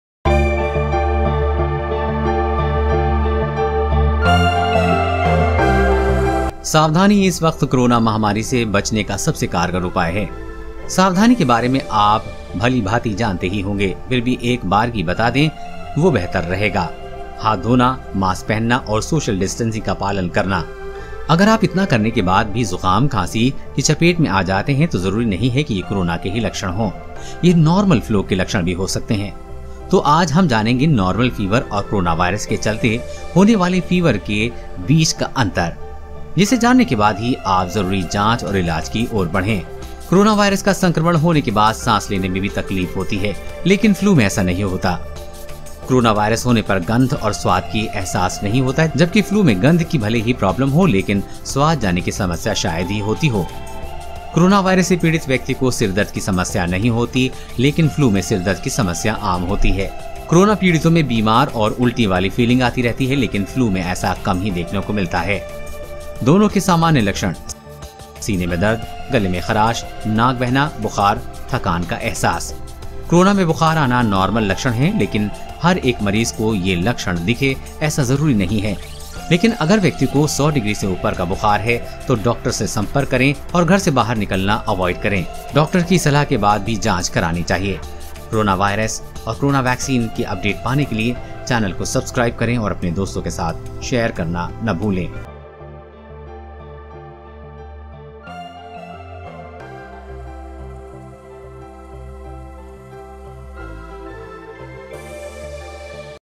इन 5 लक्षणों के आधार पर समझें नॉर्मल और कोरोना वायरस बुखार में अंतर
Corona Virus New Symptoms : Health Understand The Difference Between Normal Fever And Corona Virus Fever Based On These 5 Symptoms | Corona Virus Case Today Update | Corona Vaccine Latest News